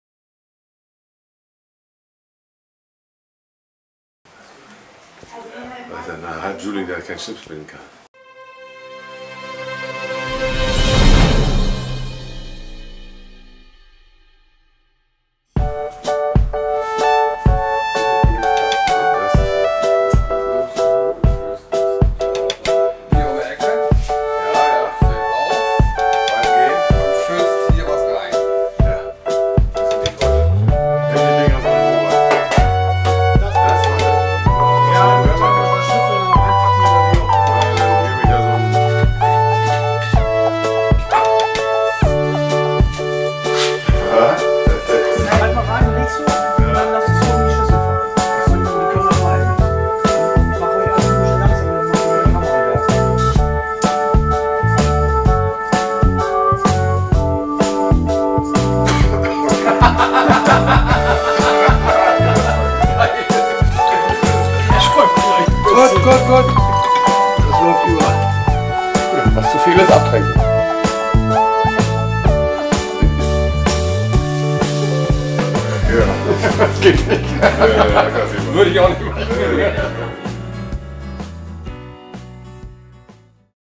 23.03.2019 - "GIV MI MOL DEN SCHLUCK, DEI DEI BULLEN HÖLPT!"
Seit 1949 ist das Restitutionsfluid, besser bekannt unter dem Namen "Bullenschluck", ein Begriff in Sulingen. Die Geschichte dazu wurde heute von uns nachgestellt.

Vielen Dank an Jens Dunker und alle Beteiligten.